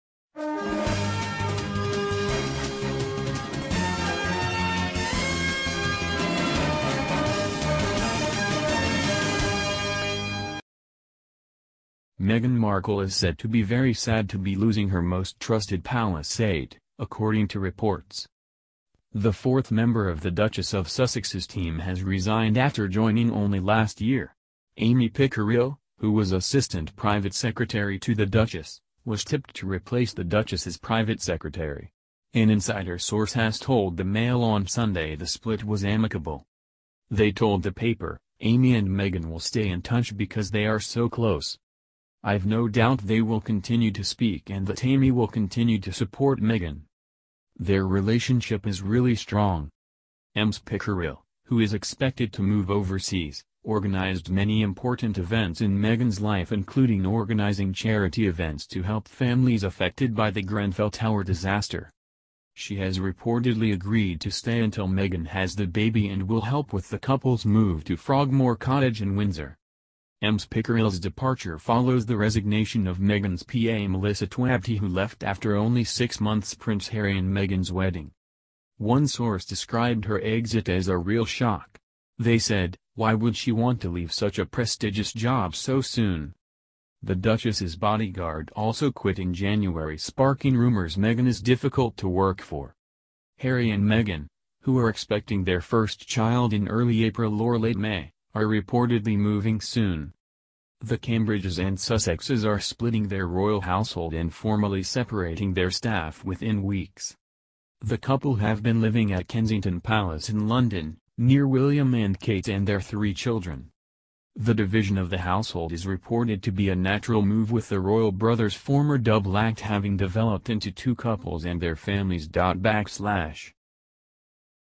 REVEALED: What Meghan Markle and departing aide's relationship was REALLY like
MEGHAN MARKLE is said to be “very sad” to be losing her “most trusted” Palace aide, according to reports.